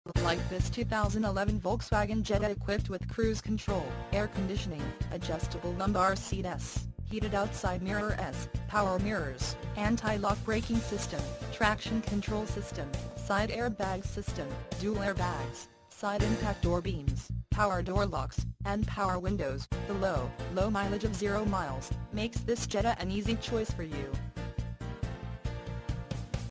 used Volkswagen Jetta NJ New Jersey 2011 located in Jersey Shore at World Family VW of Neptune
This used Volkswagen Jetta NJ 2011 is located in Jersey Shore and is offered by World Family VW of Neptune and can be compared to any used Volkswagen Jetta New Jersey :